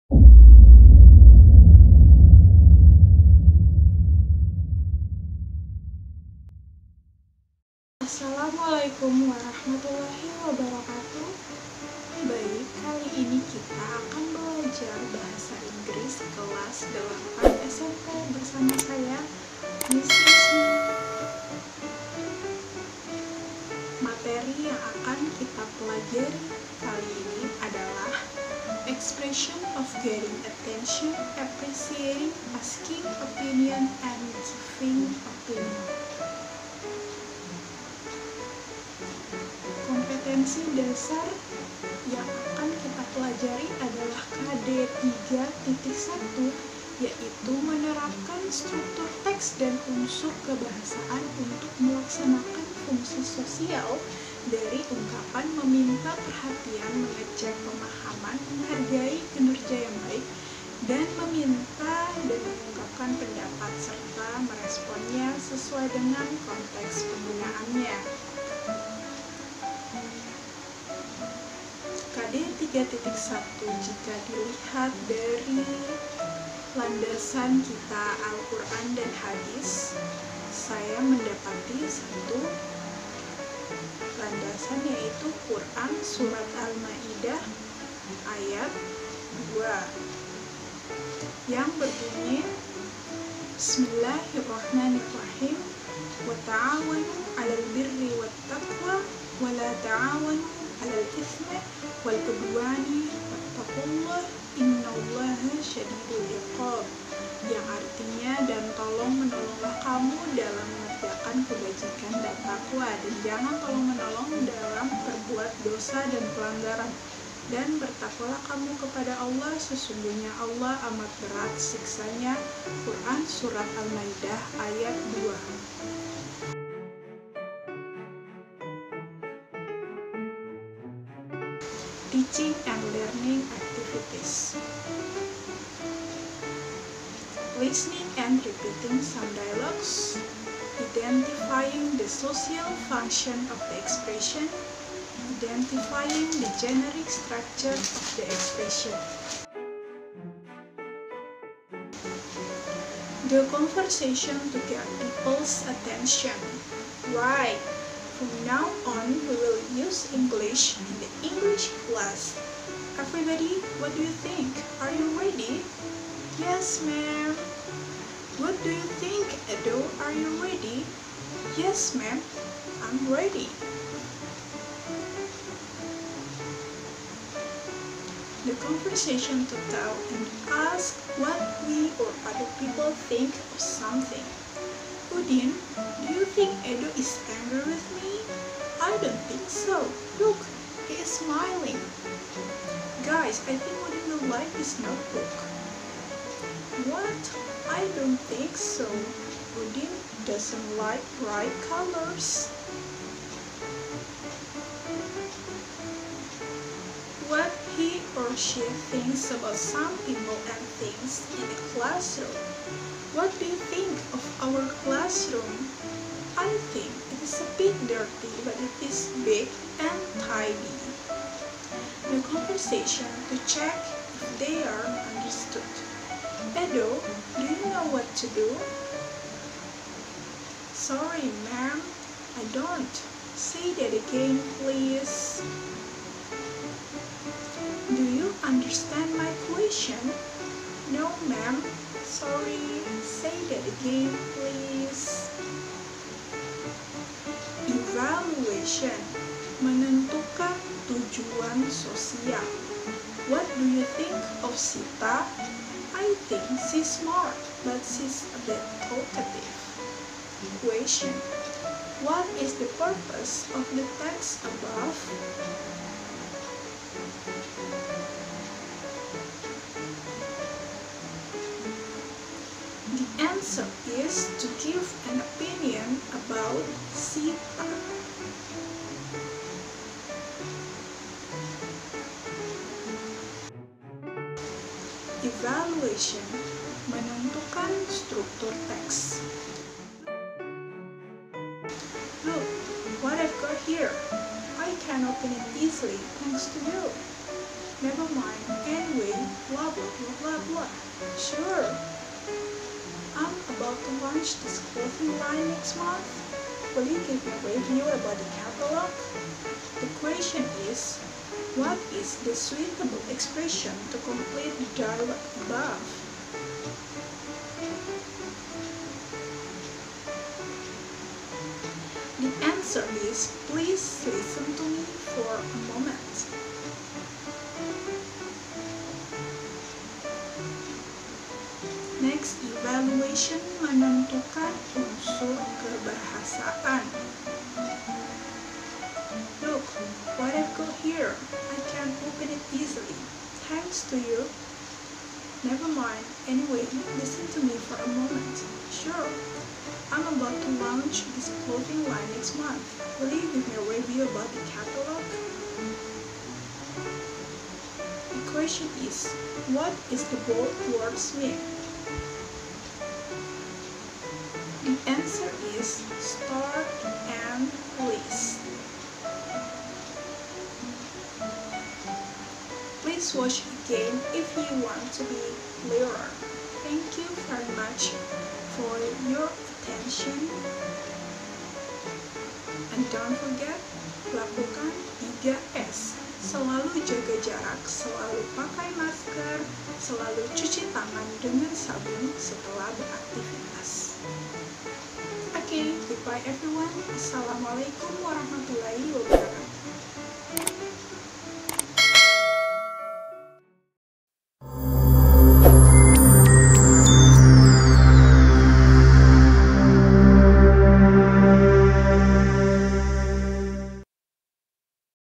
Expression of getting attention - appreciating - asking and giving opinion
Materi Pembelajaran "Expression of Getting attention, appreciation, asking and giving opinion" sesuai dengan K13 mata pelajaran bahasa inggris kelas 8 tingkat SMP/MTS, For More Quiz, click this | Jangan lupa like komen share dan subscribe channel ini ya karena subscribe itu gratissssssssssssssssssssssss | Happy watching !!